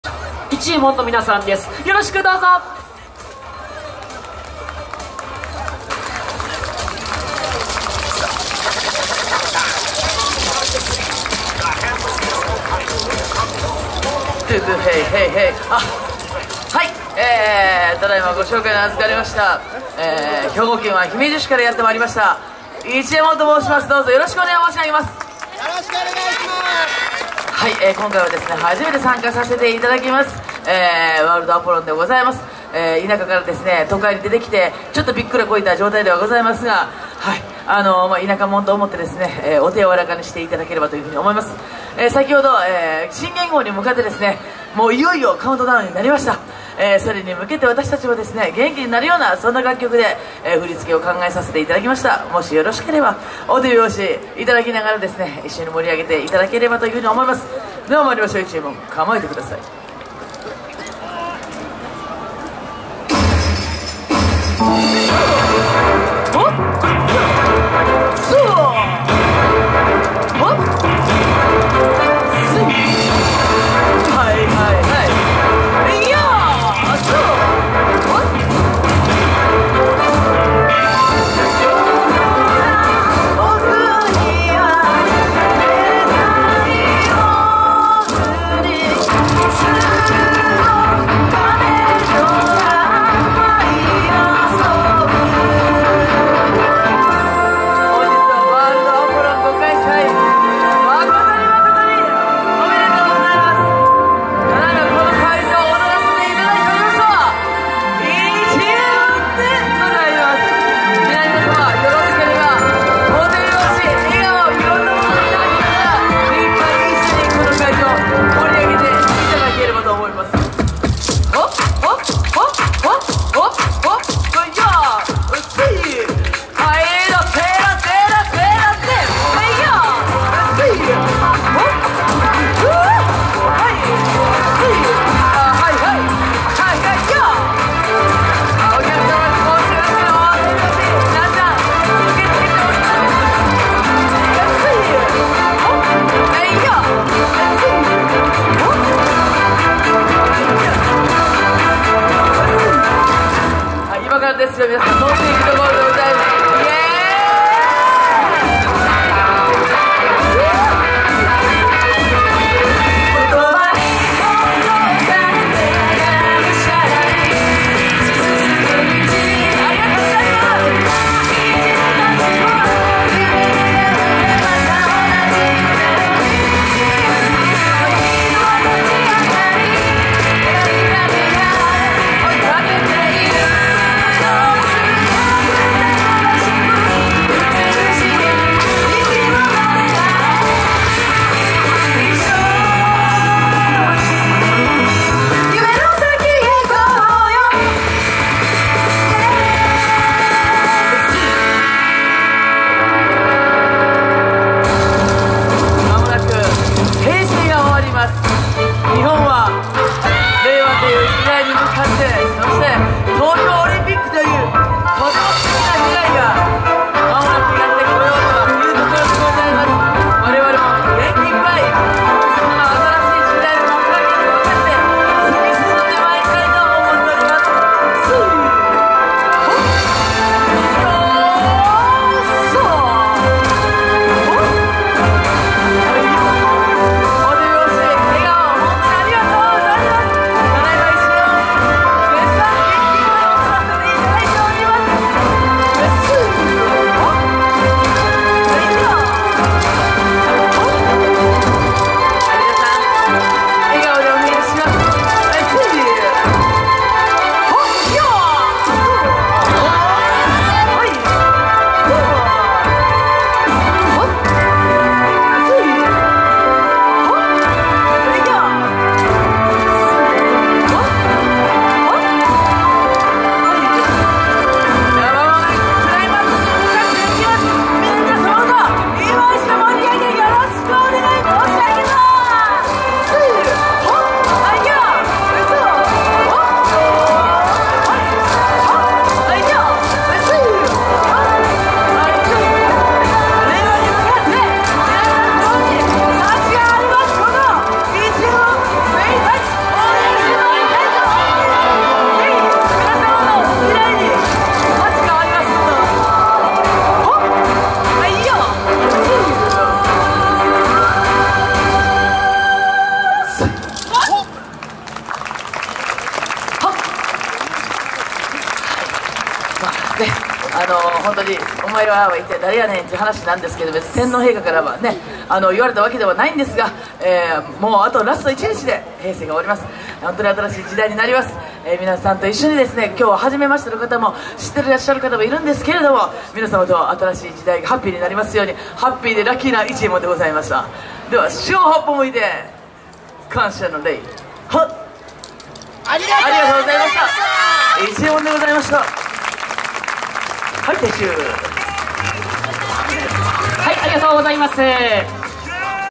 いちえもん @ Worldあぽろん (ピロティ広場) 2019-04-29T14:10
2019年4月29日(月)、大阪府住之江区で開催された「Worldあぽろん」(ピロティ広場)での「いちえもん」さんの演舞です。